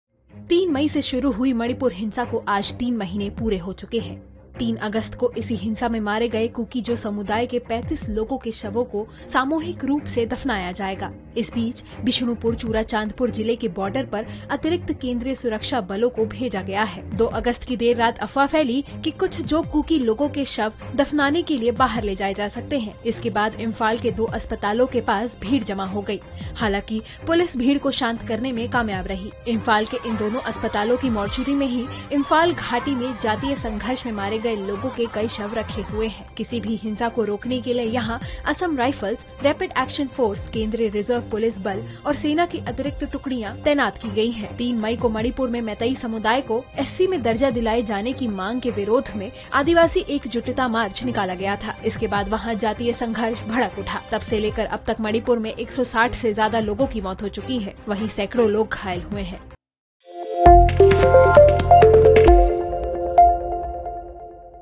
मणिपुर हिंसा के 3 महीने पूरे | Manipur completes 3 months of violence | WM HINDI
About this:   मणिपुर हिंसा के 3 महीने पूरे

मणिपुर में चल रहे मैतेई और कुकी समुदाय के बीच हिंसा जारी है और राज्य में इस दंगे में 160 से अधिक लोग मारे गए हैं। राज्य में कई लोगों के शव इंफाल के अस्पतालों में रखे गए हैं, जिनमें से 35 कुकी समुदाय के लोगों के शव सामूहिक रूप से दफनाए जाने की योजना है। कुकी-जो समुदाय का संगठन इंडिजिनस ट्राइबल लीडर्स फोरम (ITLF) तुईबोंग शांति मैदान में शवों को दफनाने की योजना बना रहा है। इस विवाद से जुड़े बॉर्डर पर अतिरिक्त केंद्रीय सुरक्षा बलों को भेज दिया गया है।

इस हिंसा से जुड़ी विभिन्न विद्वेषपूर्ण घटनाओं के चलते महिलाओं ने प्रदर्शन किया और विपक्ष के सांसदों ने राष्ट्रपति से मुलाकात कर राज्य में सुरक्षा के प्रश्नों का समाधान मांगा। सुप्रीम कोर्ट ने भी इस मामले में सुनवाई की और राज्य में सुरक्षा बढ़ाने की अपील की।

मैतेई और कुकी समुदाय के बीच तनाव कम करने के लिए शवों को दफनाने के विकल्प पर चर्चा हो रही है, लेकिन दोनों समुदायों के बीच इस विषय पर असमंजस है। कुकी समुदाय ने मैतेई के इलाके में शवों को दफनाने से इंकार किया है और यह कहा है कि वे चूराचांदपुर जिला अस्पताल की मोर्चुरी में ही शवों को दफनाएंगे। मैतेई समुदाय भी इसके खिलाफ खड़ा है और कहता है कि यदि शवों को इम्फाल भेज दिया जाता है, तो वहां भी दफनाएंगे।

मैतेई और कुकी समुदाय के बीच इस दंगे से तनाव बढ़ सकता है और सरकार को समस्या का समाधान ढूंढने के लिए कड़ी मेहनत करनी होगी।

Love, Instagood, PhotoOfTheDay, Beautiful, Travel, Fashion, Food, Fitness, Nature, ThrowbackThursday (TBT)